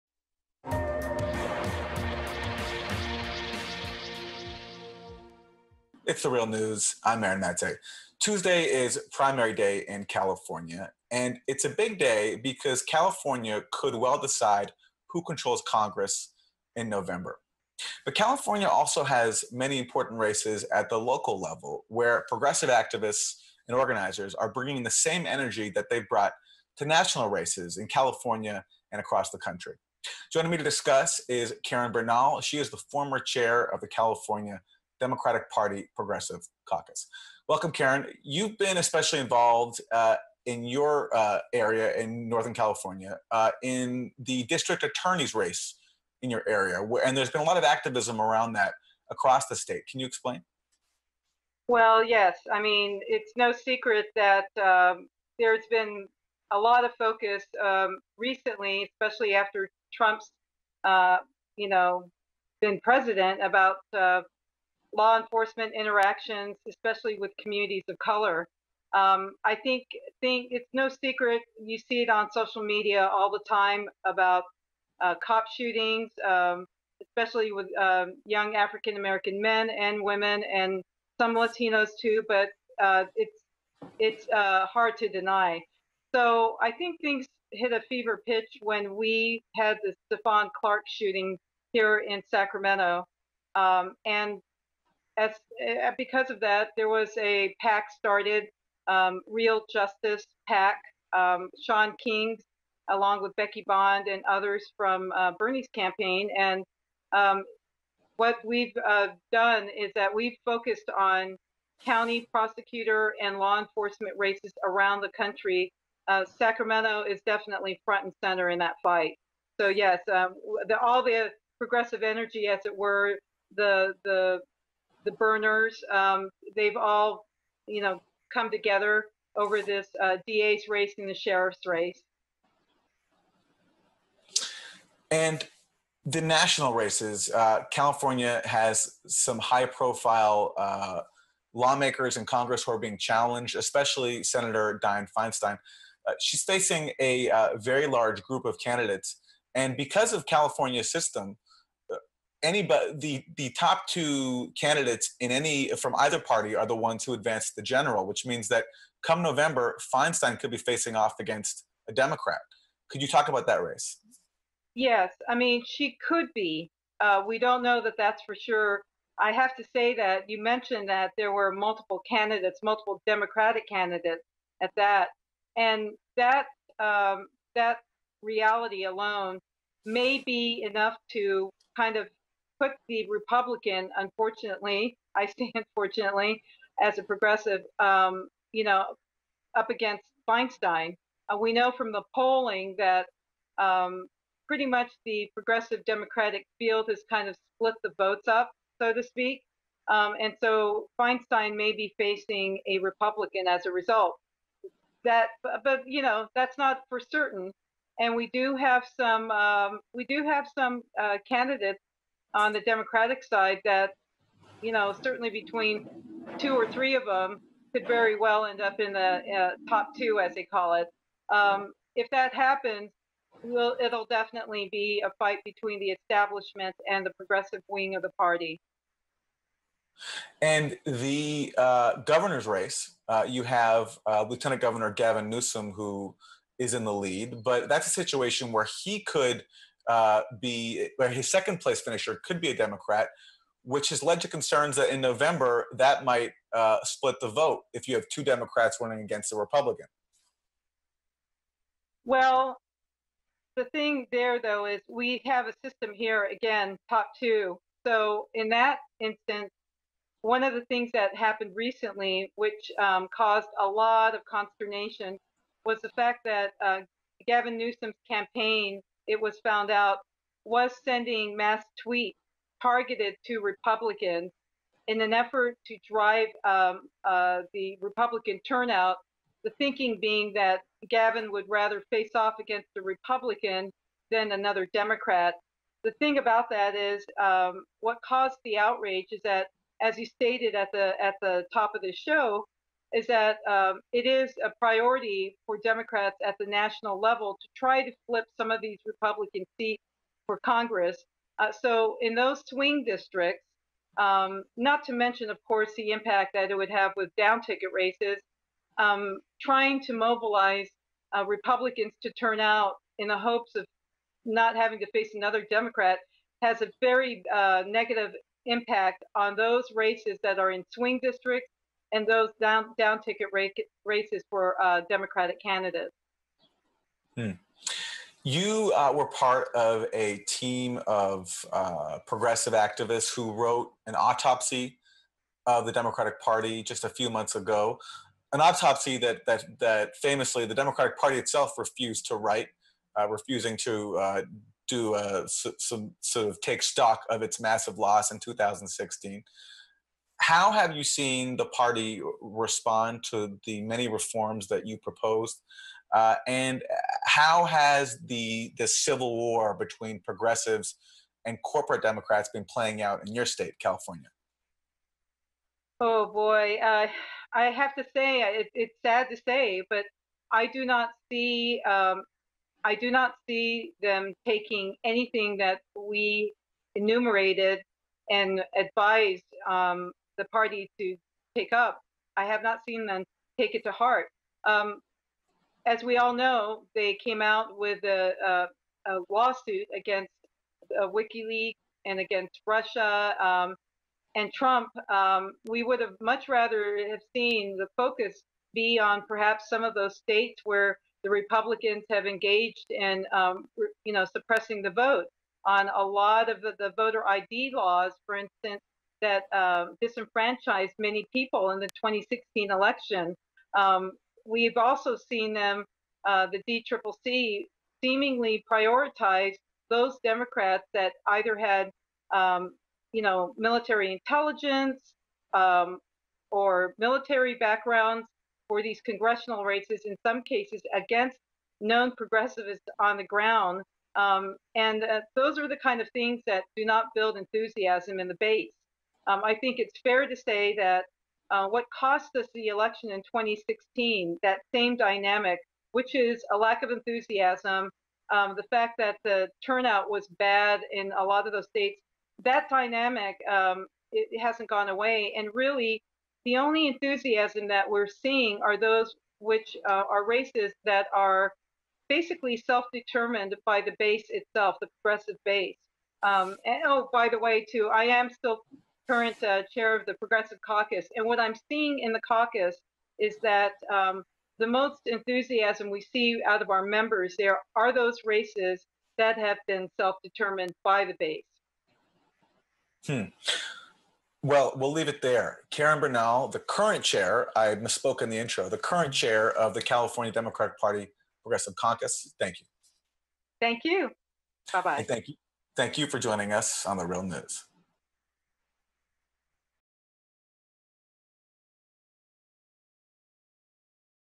California Progressives Seek Democratic Party Overhaul at All Levels
Karen Bernal, chair of the California Democratic Party Progressive Caucus, says that progressives are competing aggressively at all levels of government in the June 5th primary